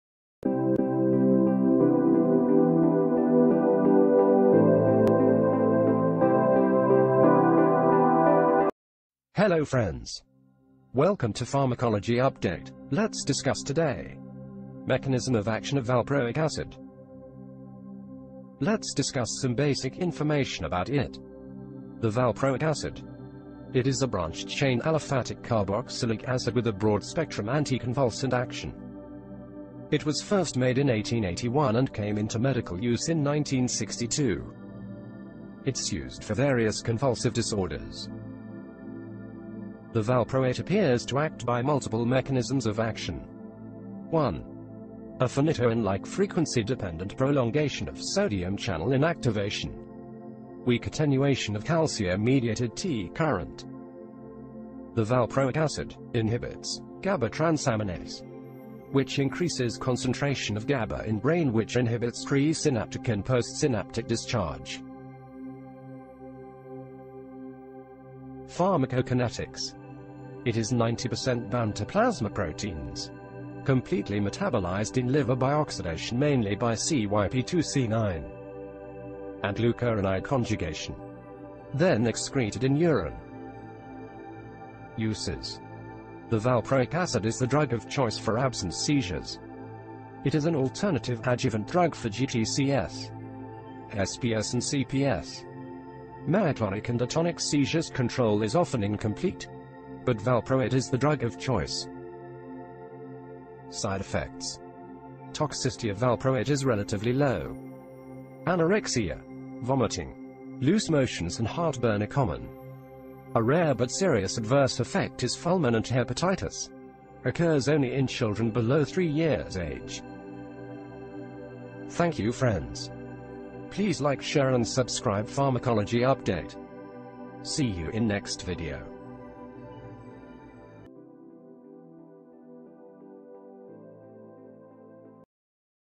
Anti Epileptic Drugs: Sodium Valproate
Starting New Series on Anti Epileptic Drugs.